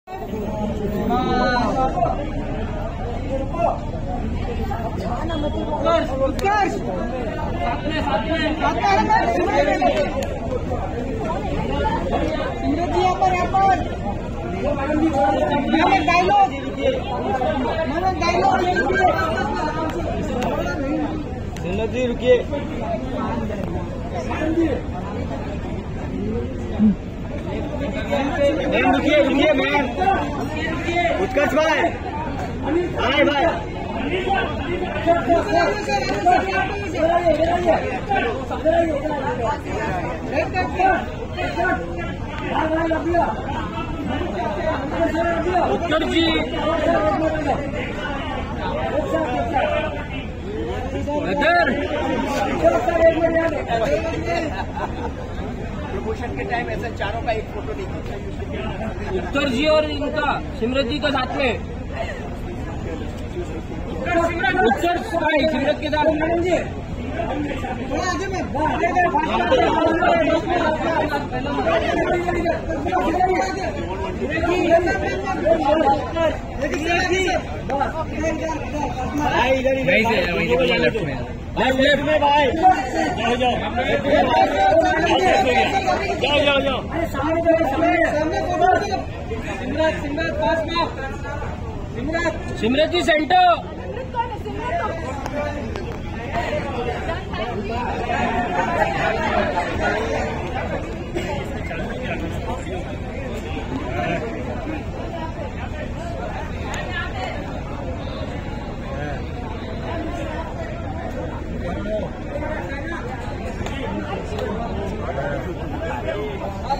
Simrat Kaur Utkarsh Sharma Anil Sharma Gadar 2 looking together at International Iconic Awards
Simrat Kaur Utkarsh Sharma Anil Sharma villain Manish Wadhwa Gadar 2 looking together at International Iconic Awards